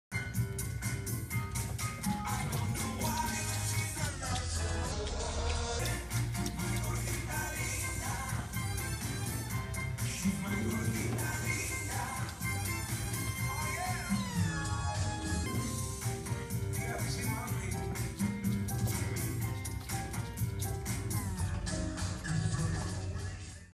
Julia Gorchakova Swish!
Julia Gorchakova doing Swish of the Week in Hematite and Crystal Latin Dress.